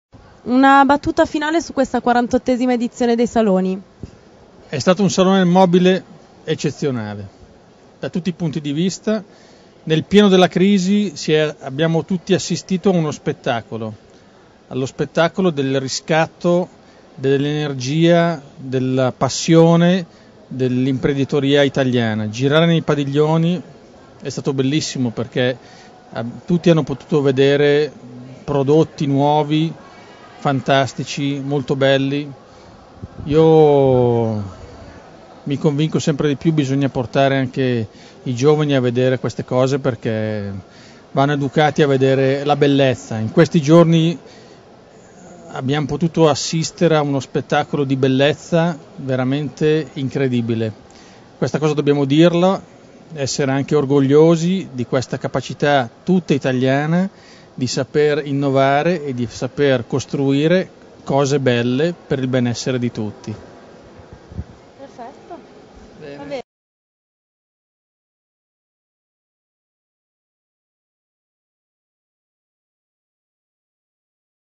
Intervista a Giovanni De Ponti
Intervista a Giovanni De Ponti in occasione del Salone Internazionale del Mobile 2009